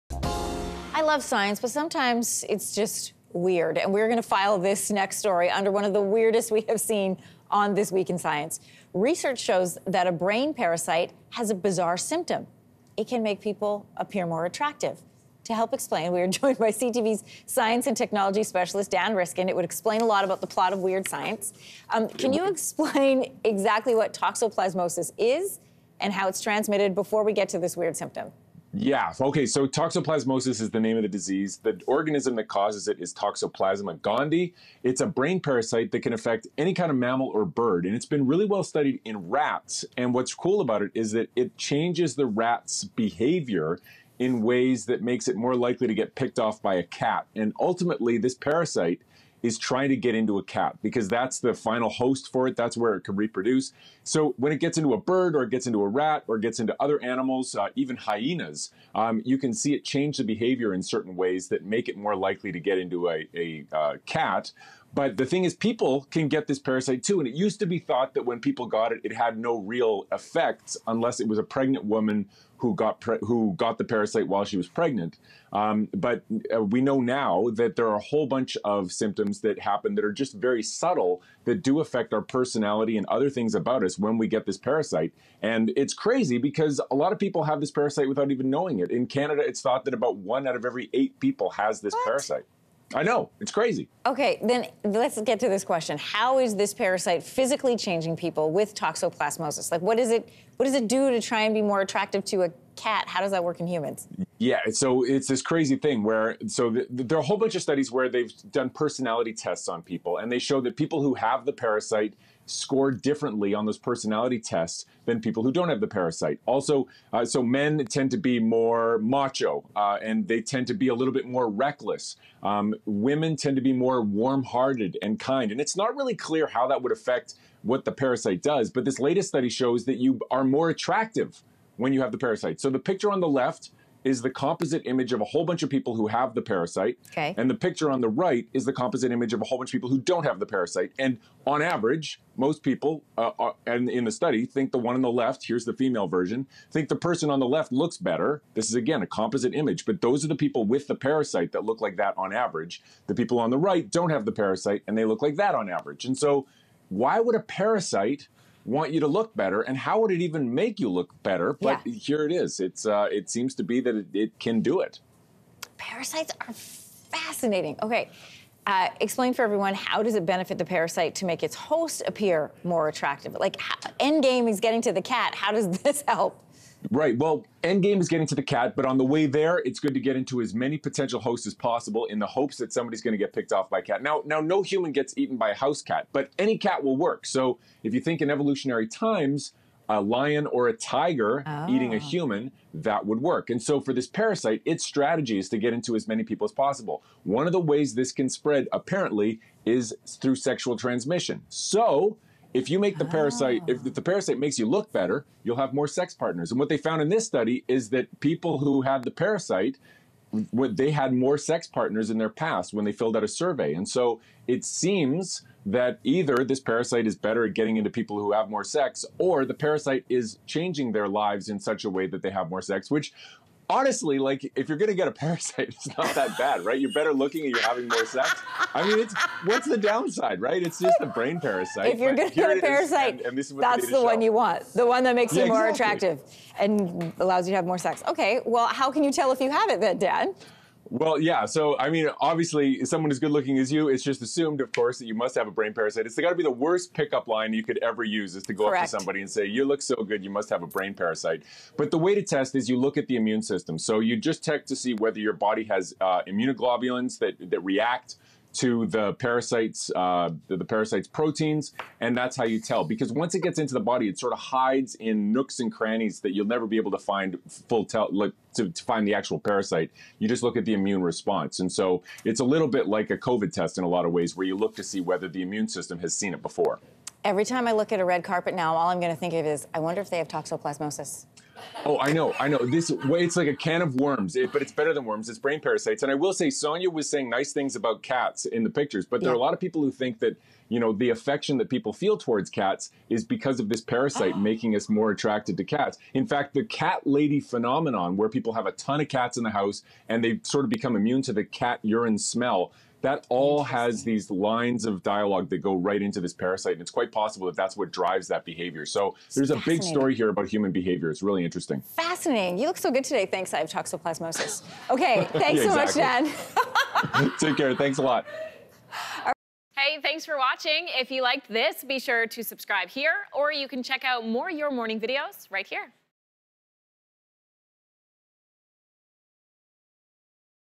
Brain parasite makes people appear more attractive | Your Morning
CTV’s Science and Technology specialist Dan Riskin tells us about the new research.